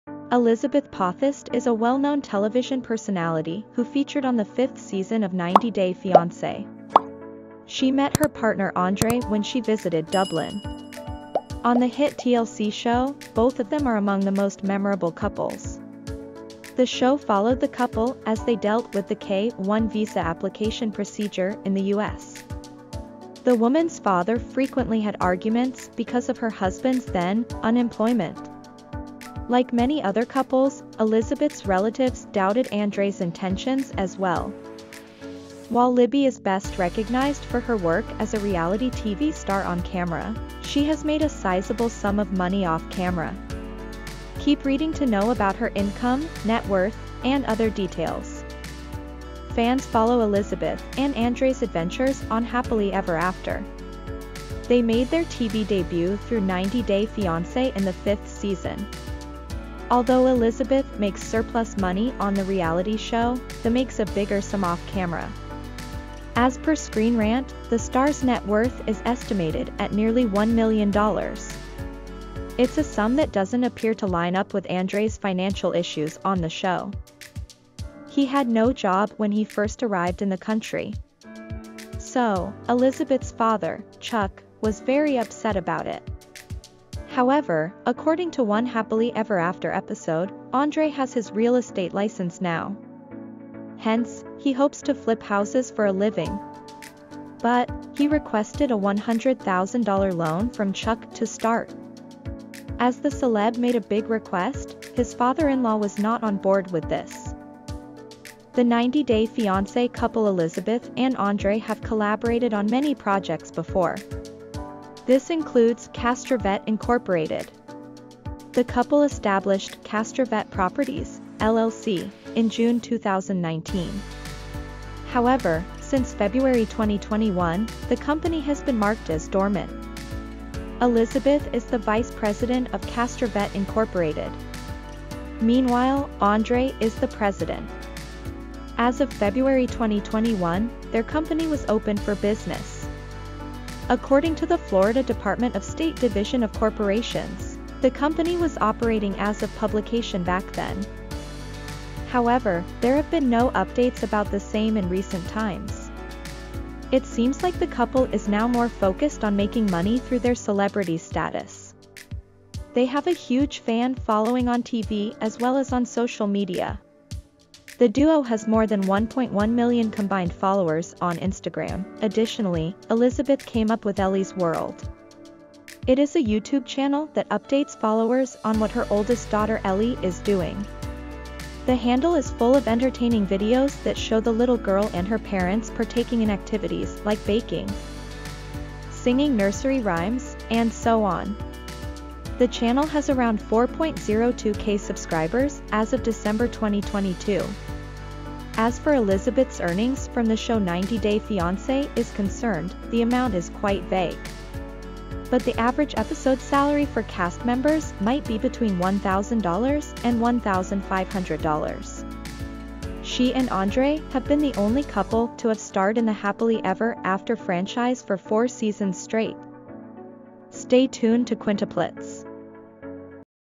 90 Day Fiance: Elizabeth Potthast Is The RICHEST’Her Net Worth Is More Than You Thought!?
90 Day Fiance: Elizabeth Potthast Earns A Fortune!

Fans follow Elizabeth and Andrei’s adventures on Happily Ever After. They made their TV debut through 90 Day Fiance in the fifth season. Although Elizabeth makes surplus money on the reality show, the makes a bigger sum off-camera. As per Screenrant, the star’s net worth is estimated at nearly $1 million! It’s a sum that doesn’t appear to line up with Andrei’s financial issues on the show. He had no job when he first arrived in the country. So, Elizabeth’s father, Chuck, was very upset about it. However, according to one Happily Ever After episode, Andrei has his real estate license now. Hence, he hopes to flip houses for a living. But, he requested a $100,000 loan from Chuck to start. As the celeb made a big request, his father-in-law was not on board with this.